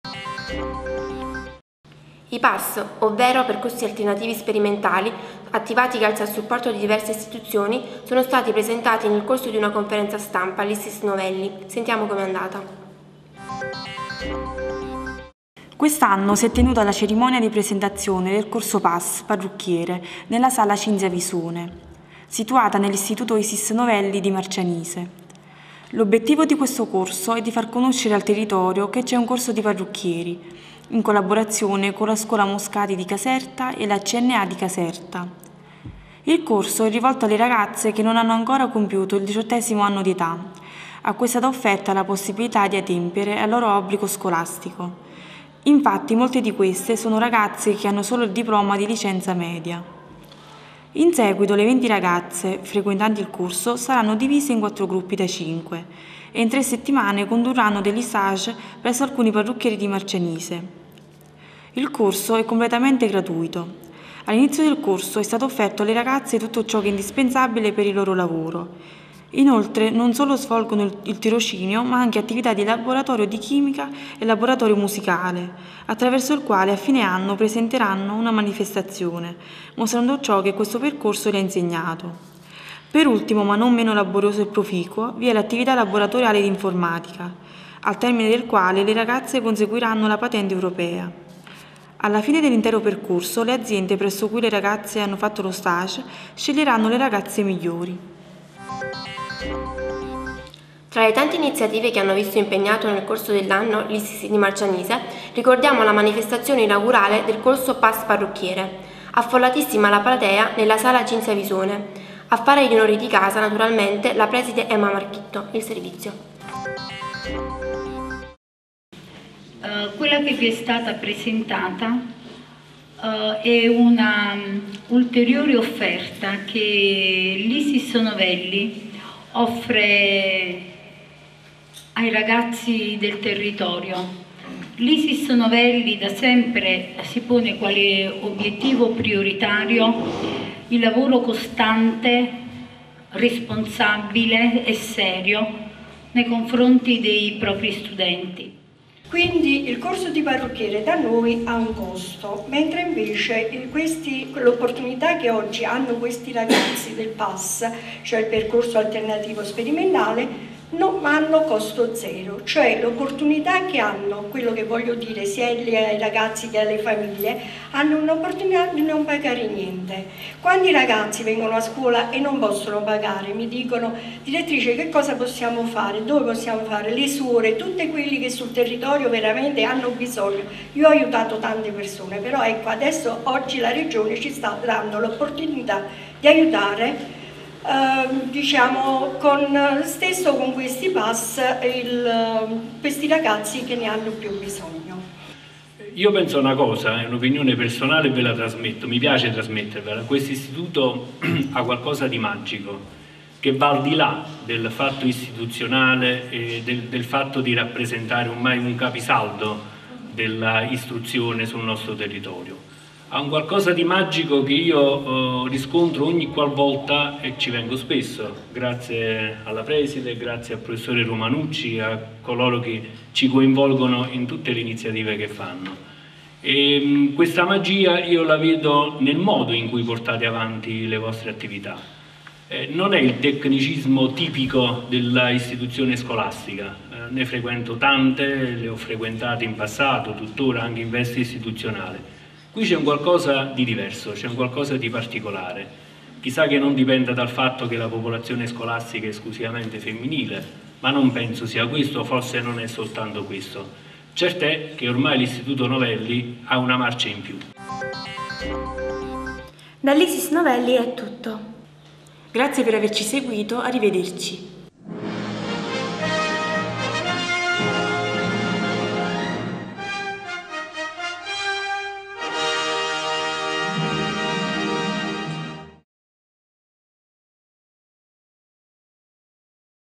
tg novelli 3 - 3
Le allieve del noto istituto ISISS GB Novelli di Marcianise insieme alla giornalista Beatrice Crisci e al filmaker Luigi Nappa hanno realizzato un telegiornale sulle attivita' svolte durante l'anno scolastico 2009/10.